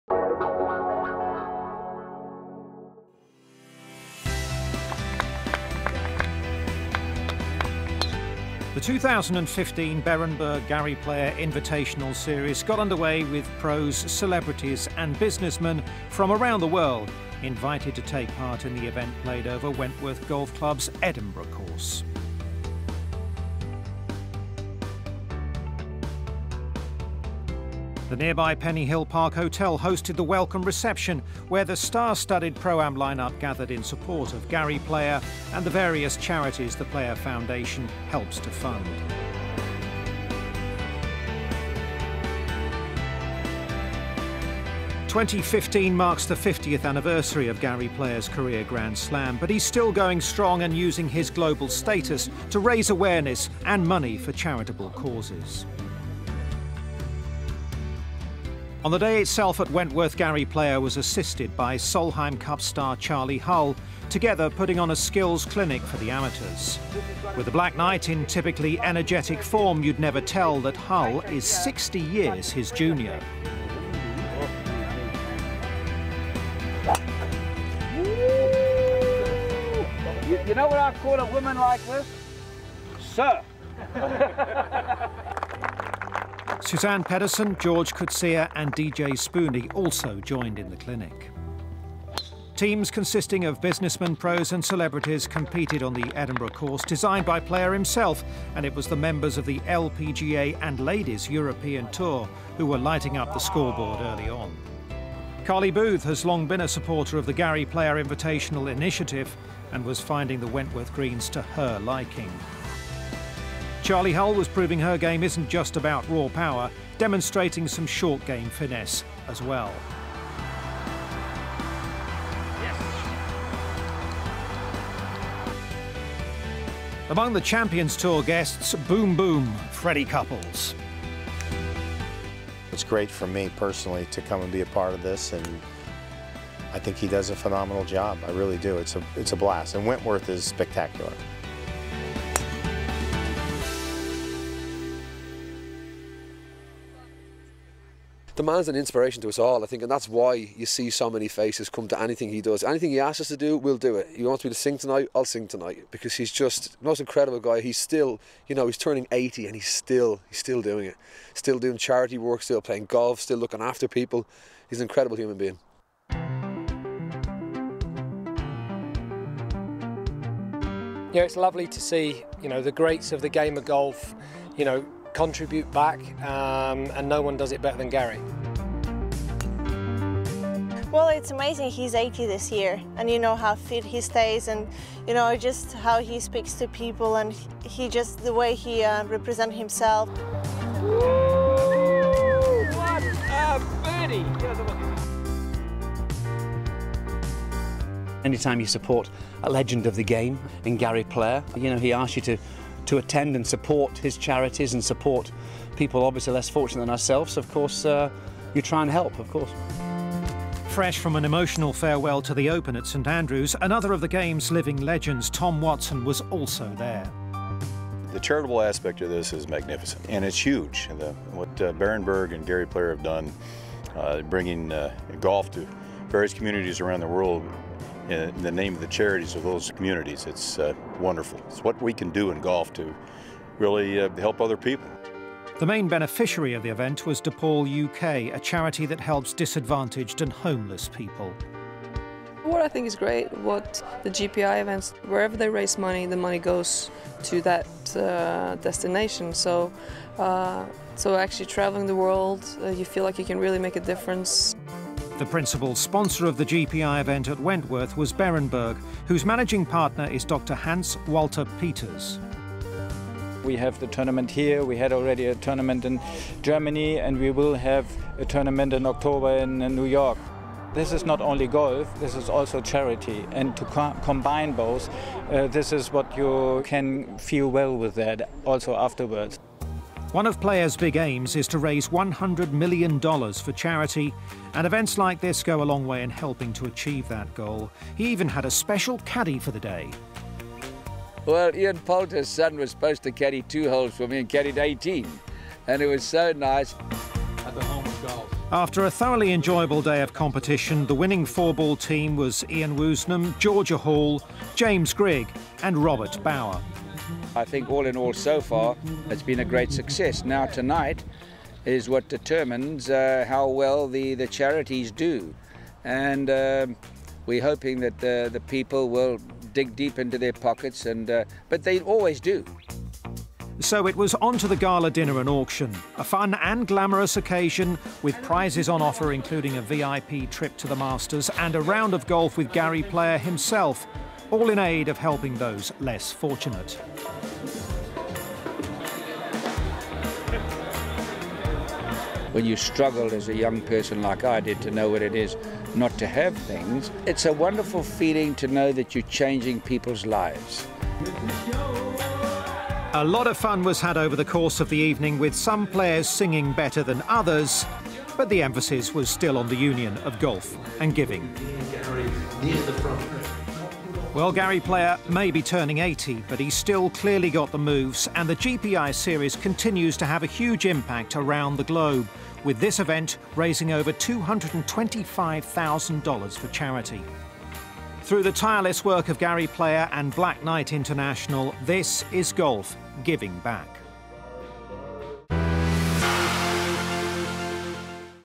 GW Inside The Game: Gary Player Invitational - 2015
Behind the scenes at the recent Invitational event hosted by legend of the game Gary Player, an event which raised over $225,000 for charity